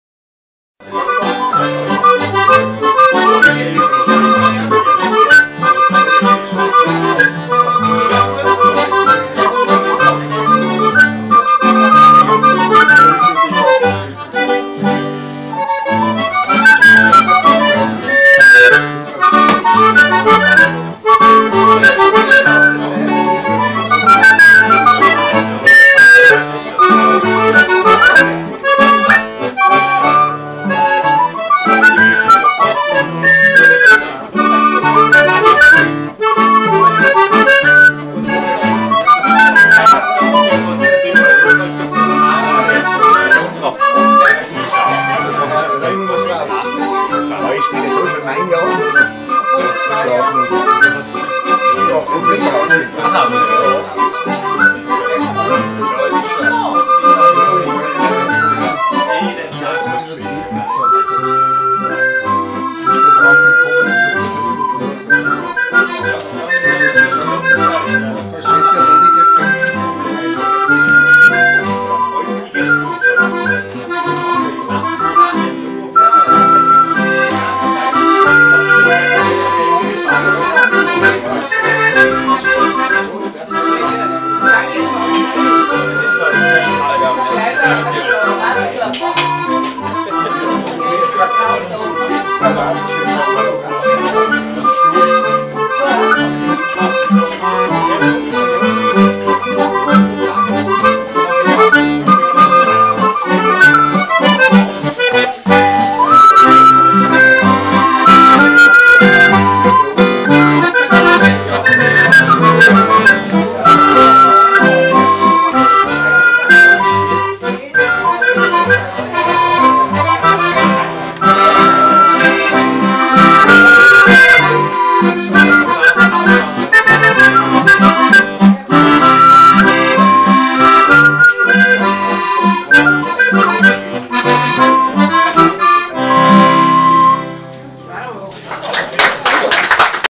Brazer Stubnmusig
Edith, Kurt und Clemens spielen beim Stammtisch in Pettneu am Arlberg